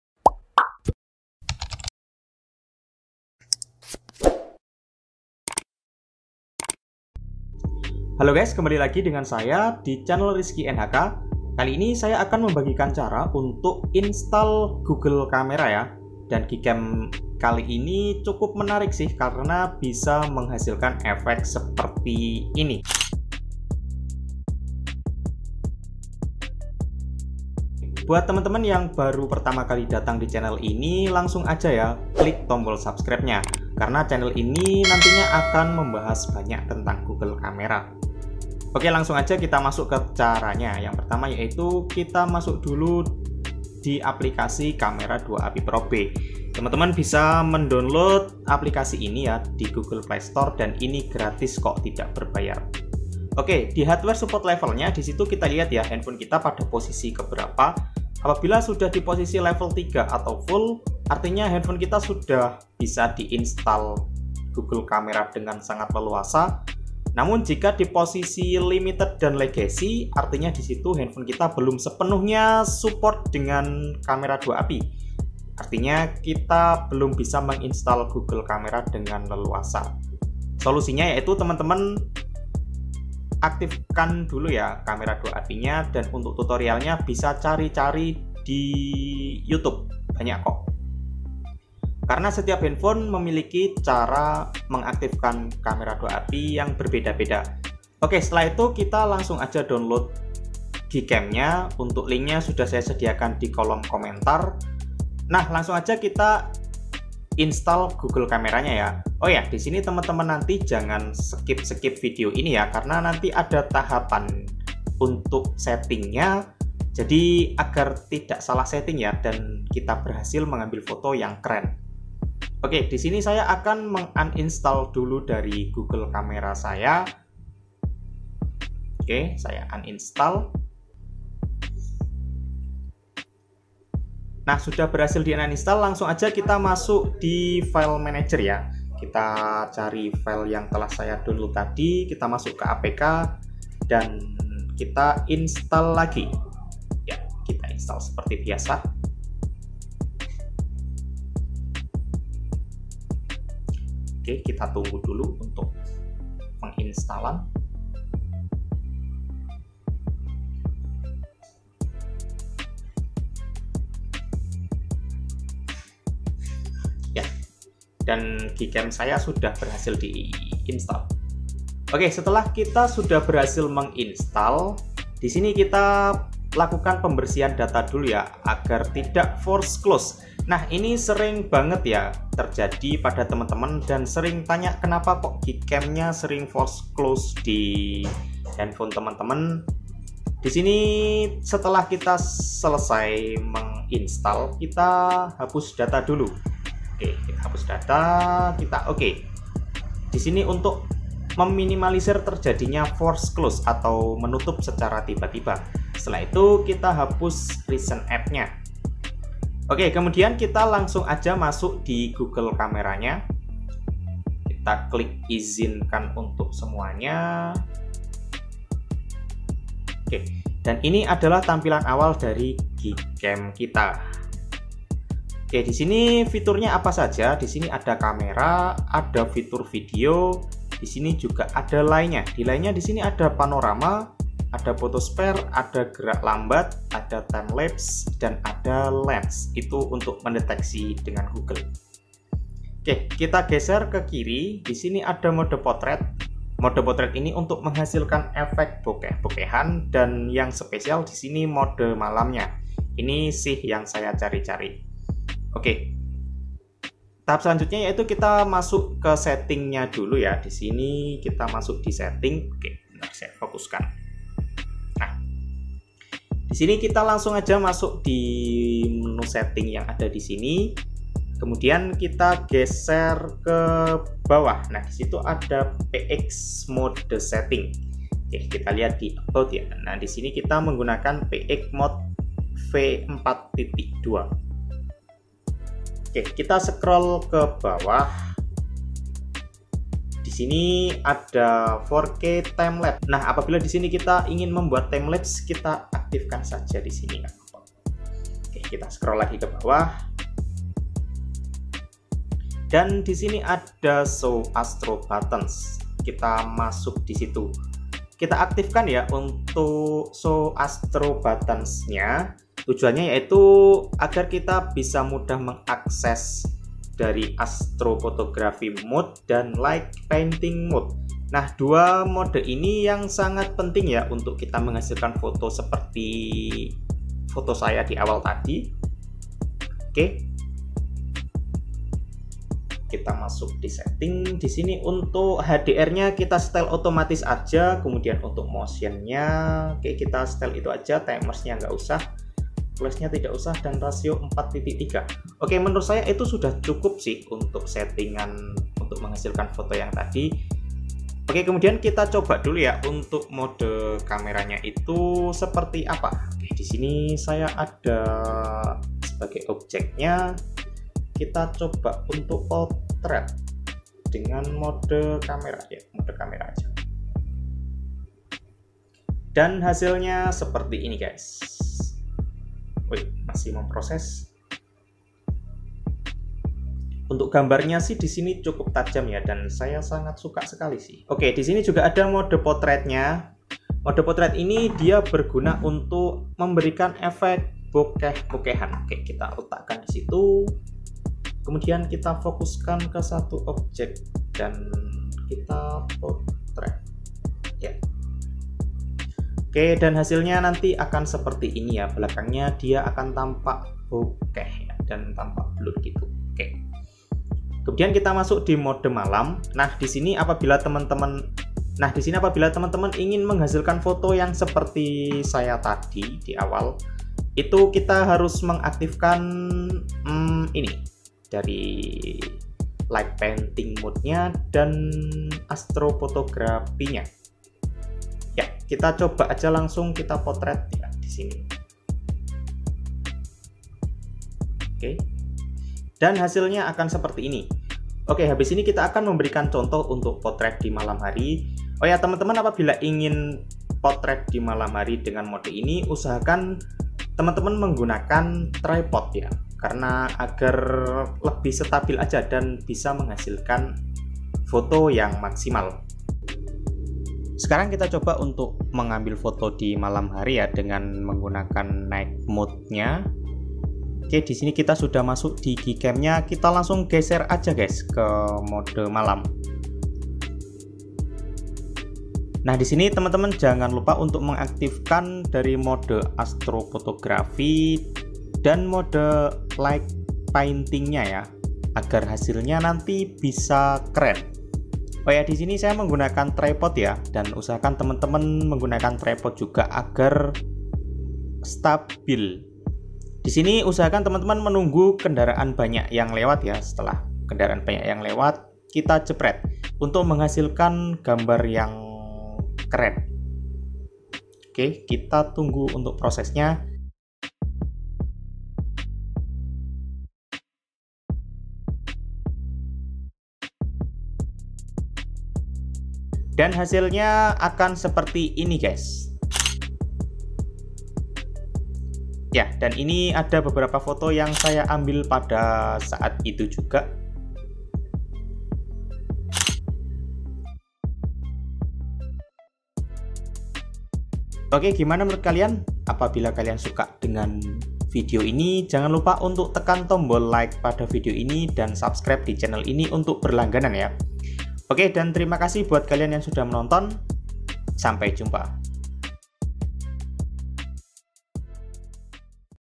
GCAM PX V4.2 MANTAP BETUL ! | BEST GCAM
Video Ini kita membahas  GCAM PX V4.2 MANTAP BETUL !  | BEST GCAM
Divideo ini saya menggunakan HP REDMI 8, android 10